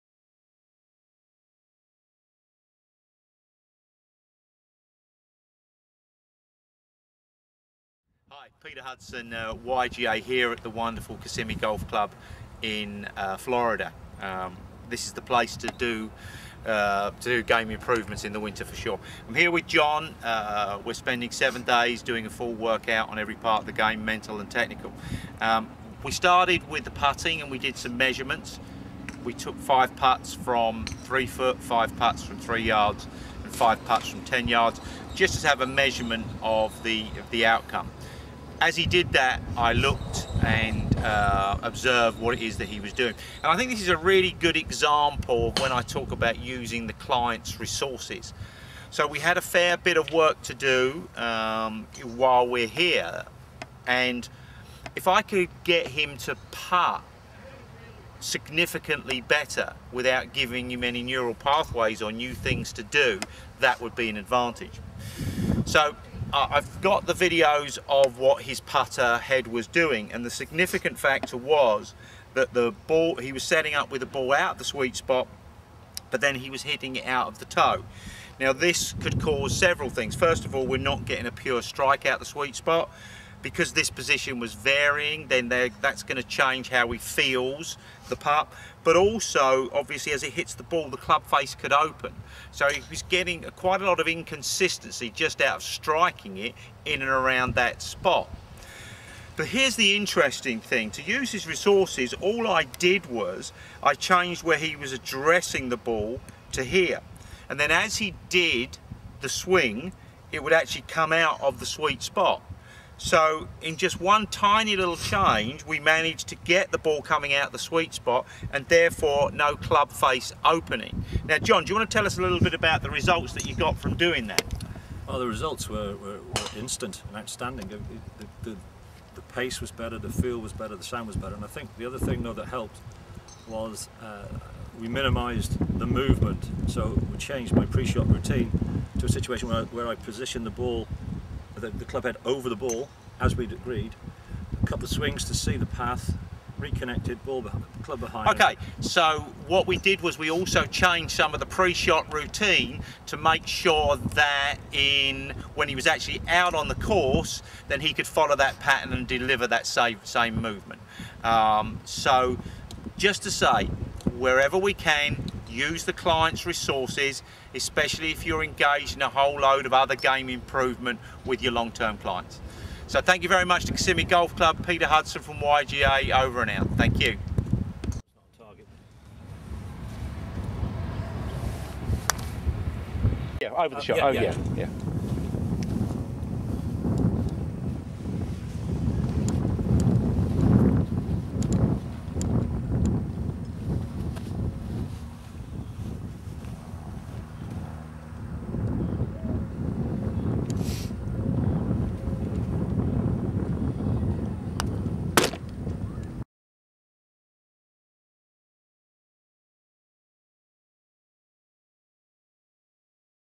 Demo: Using clients resources - kissimmee YGA
YGA coaching example

Using the clients resources

January 2015 Kissimmee Golf Club
https:peterhudsongolf.com  and claim your Questionnaire